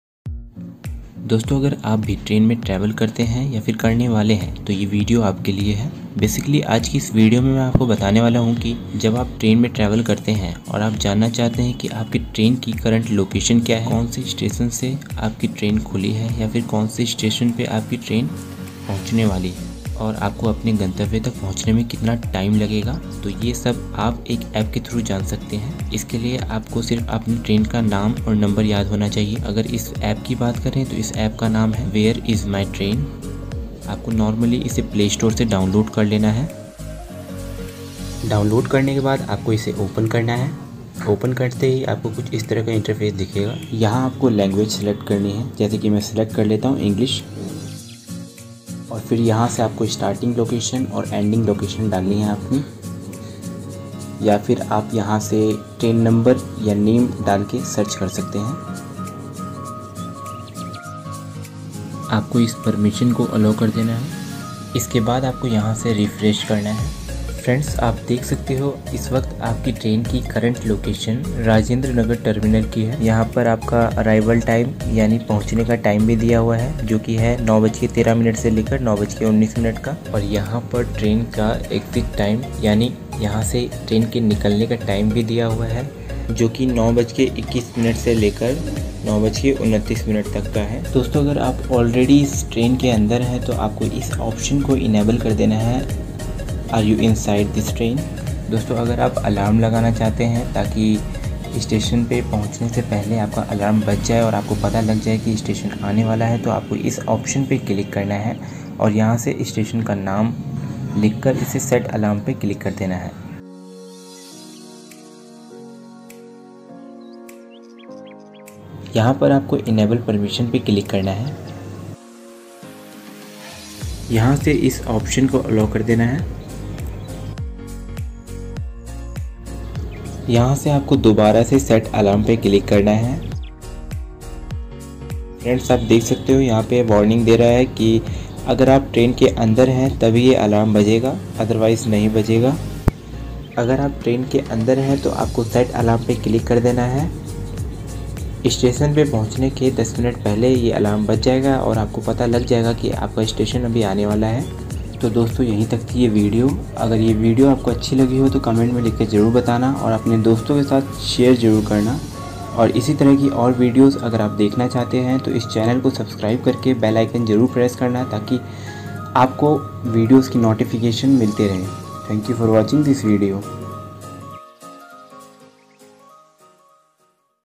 train ki live location kaise pata kare | how to check train live location
train ki live location kaise pata kare

dosto is video me maine bataya hai ki aap apne train ki live location kaise check kar sakte hai